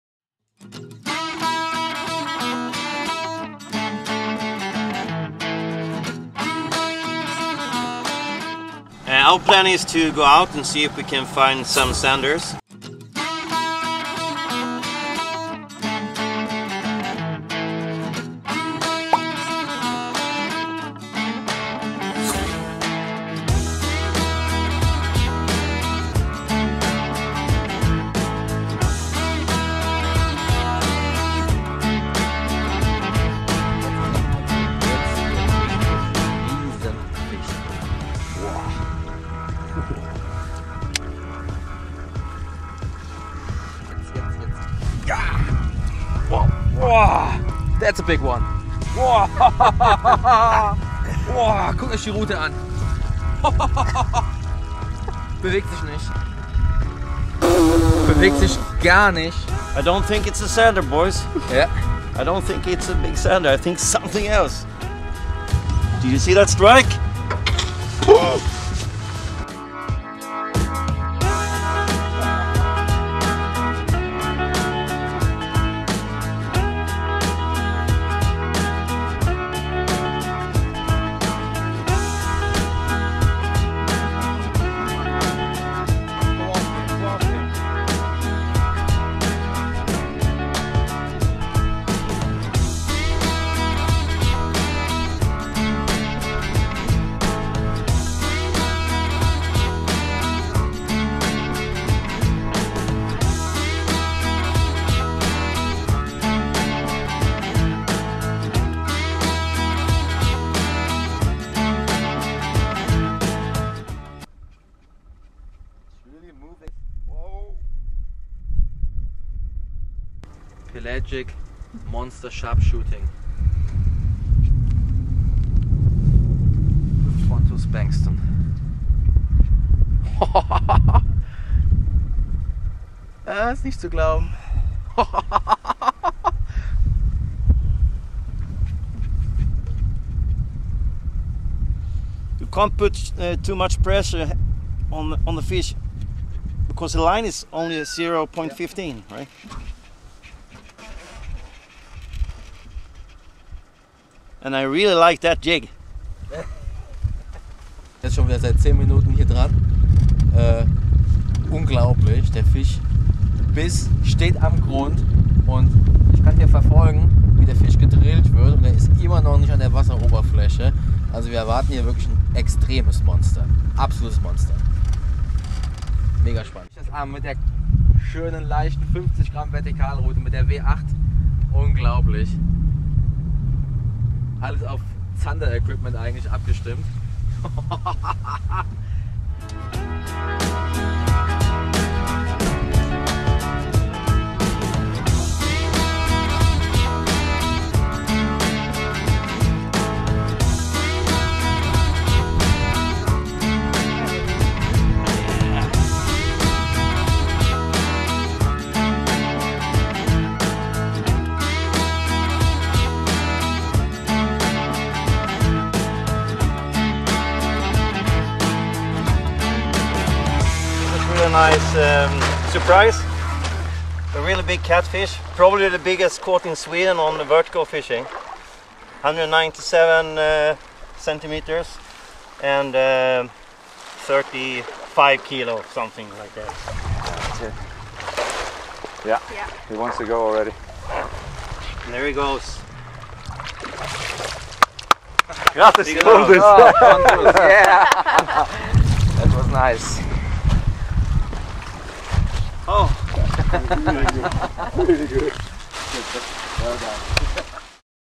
Pelagiskt vertikalfiske efter gös resulterade i en 2 meters monsterfisk.
Magisk svensk jättemal på nästan 2 meter och ca 35-40 kg tagen på pelagiskt vertikalfiske. En fantastisk bonusfisk som fångades på video vid en inspelning av prickskytte efter gös, gädda och röding av ett filmteam åt tyska Garmin. Fisken fångades på ett Westin W8 spö -52 g, en Westin 6" Twin-teez jig och 0.15mm Sufix 832 lina.
Fighten tog 45 minuter innan fisken kunde landas.
Det tyska filmteamet ringde under tiden en vän som jobbat i Italien som malguide i många år. Han bodde nu i området och kom till undsättning och hjälpte till med att säkert återsätta den fantastiska fisken som troligtvis hade en ålder på ca 100år enligt malforskare i Sverige.
Obs, malen fångades som bifångst under riktat fiske efter gös. Viktigt, mal är fredad i Sverige.